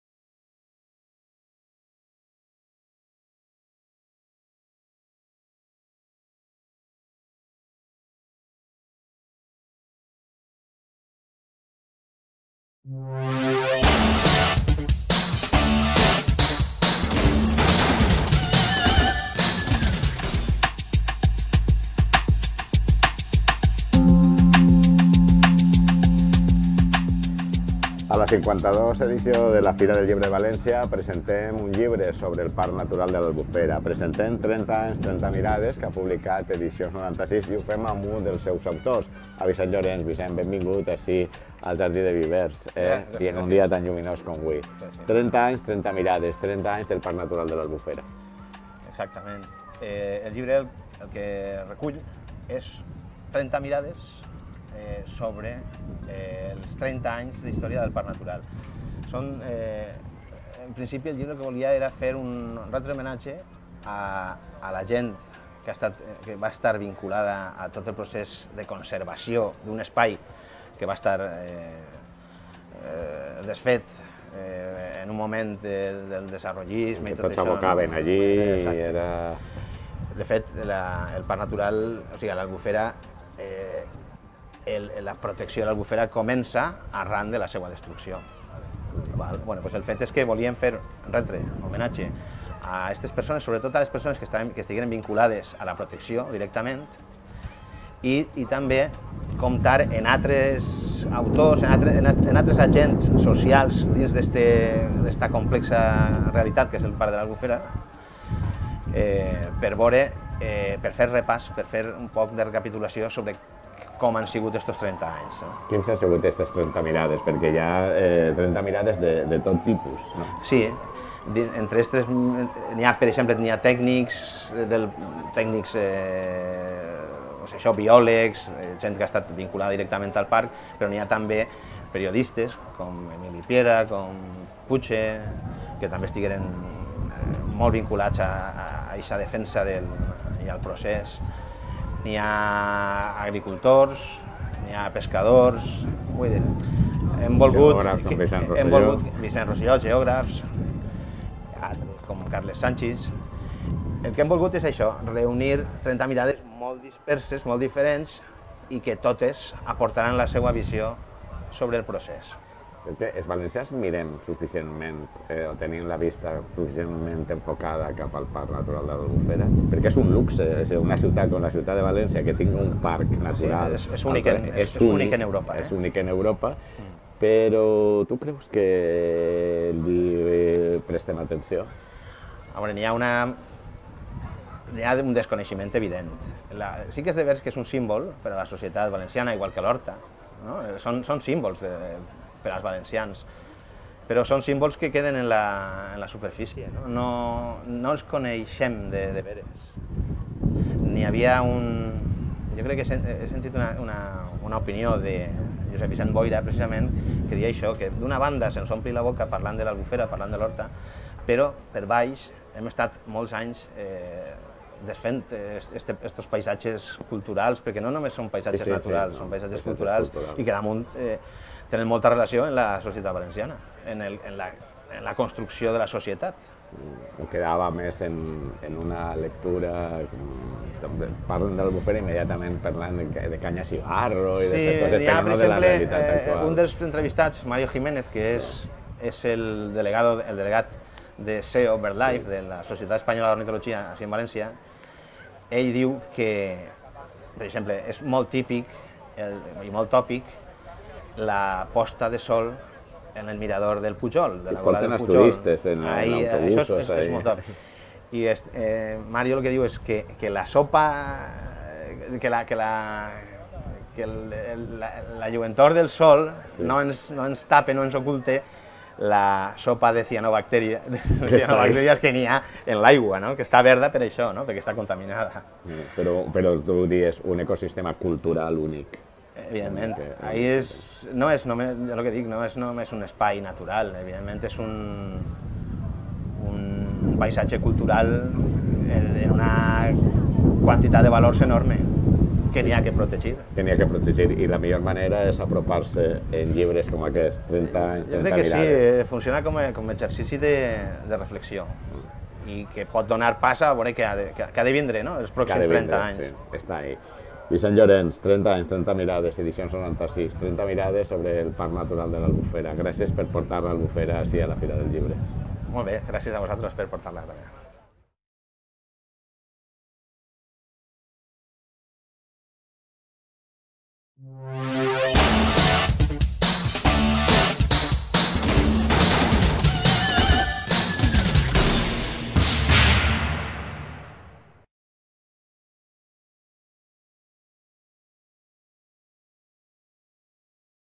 Fira del Llibre 2017 VICENT LLORENS “Trenta anys, trenta mirades” Edicions 96
A partir de trenta converses amb el mateix nombre de persones —en algun cas a duo— vinculades a l’Albufera, Vicent Llorens i Bosco Dies, efectuen un repàs dels trenta anys d’història d’aquest parc natural. El llibre és un homenatge als qui van fer possible la seua declaració l’any 1986 i als qui durant tot aquest temps han contribuït d’alguna manera a la seua protecció. Trenta anys, trenta mirades és també la crònica d’aquestes tres dècades explicada per alguns dels protagonistes i testimonis d’un procés que ha estat farcit de dificultats, embolcallat de llums i d’ombres.